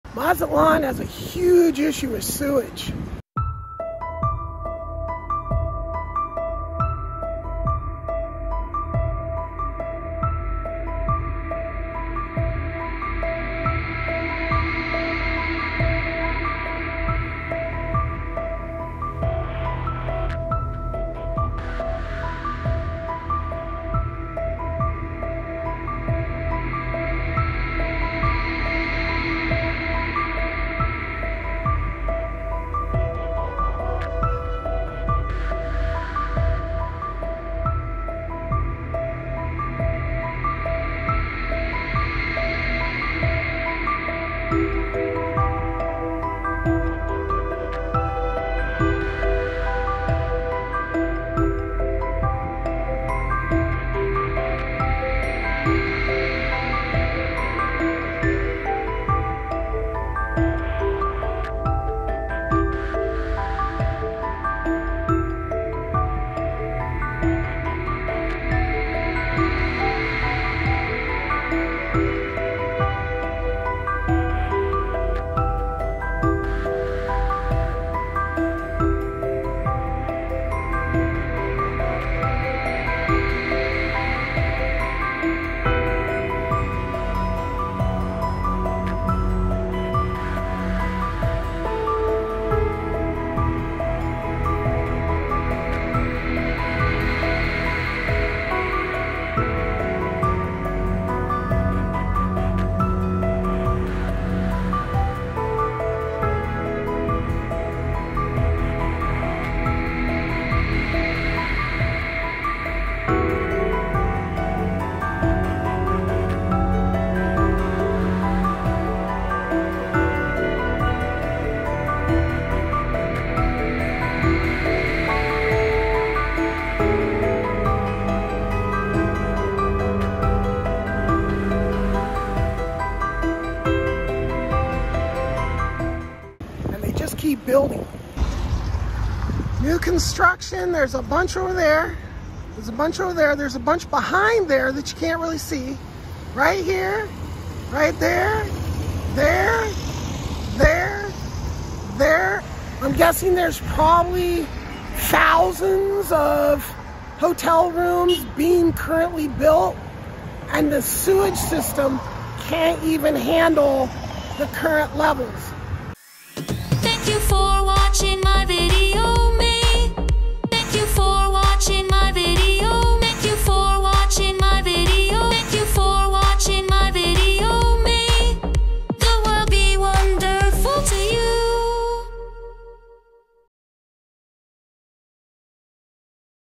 Mazatlan Mexico has a Sewage problem
I got a scary sinus infection from what I guess were aerosolized sewage particles I inhaled while walking by a sewage blowout.  The war on drugs affects the world in weird ways. Being a politician is hazardous to your health in Mexico. So you don't end up with the best options.

As mayor of Mazatlan, I will build new sewage/runoff systems at the expense of the travel corporations building all the rooms. Viva La Mexico!